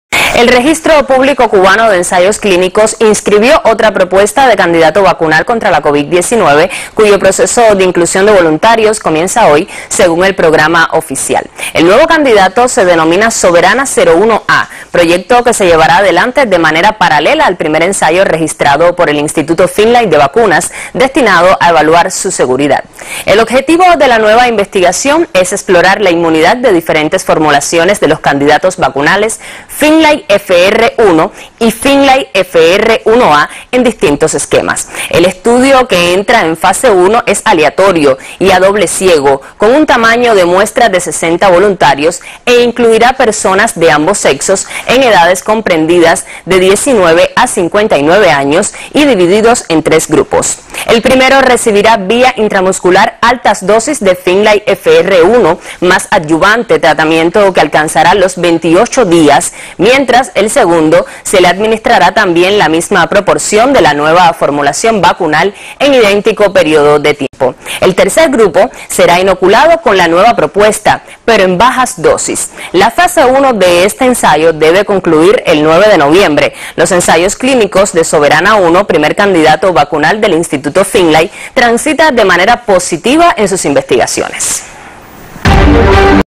Cuba registra nuevo ensayo clínico de candidato vacunal contra la COVID-19
El Registro Público Cubano de Ensayos Clínicos publicó un estudio que incluye un nuevo ensayo clínico con el "candidato vacunal profiláctico" contra la COVID-19, llamado, de manera oficial, "FINLAY-FR-1A", el cual pasaría a ser SOBERANA 01A.